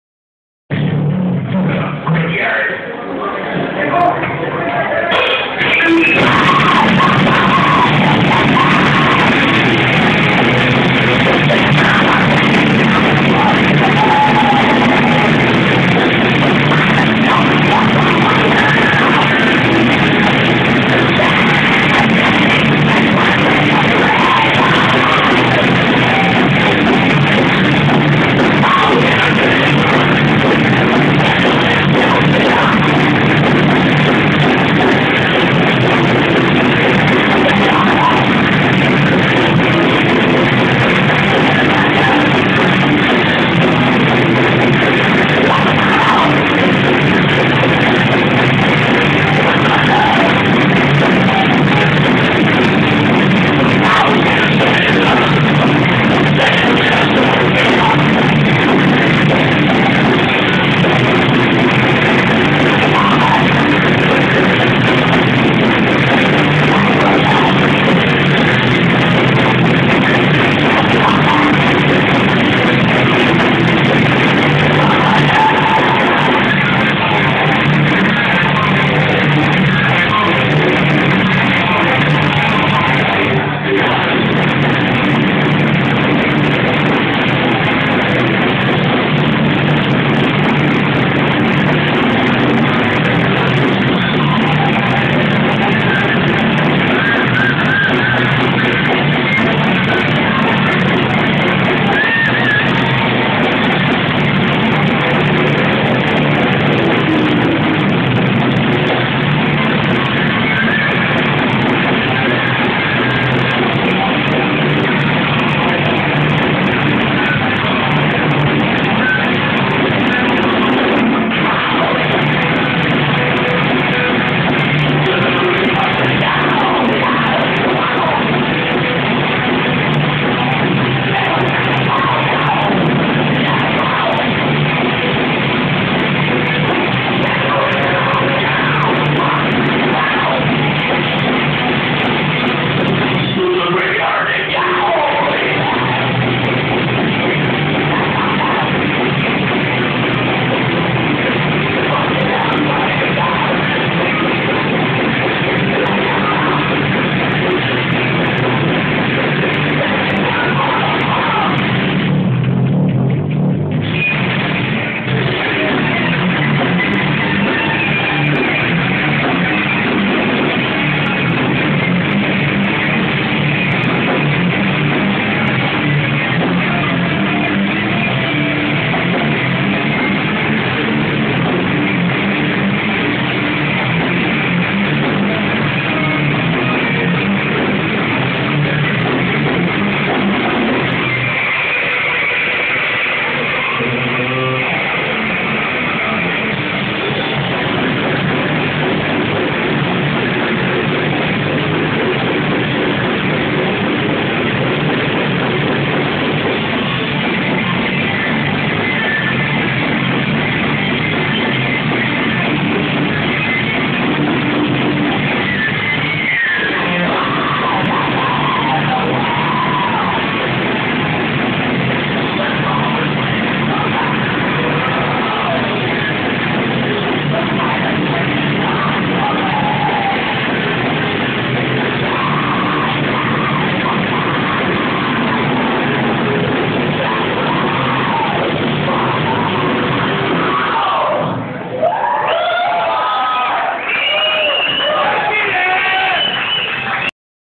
piraña en el black dog
los piraña dando una probadita de lo que va a ser su nuevo material estén pendientes.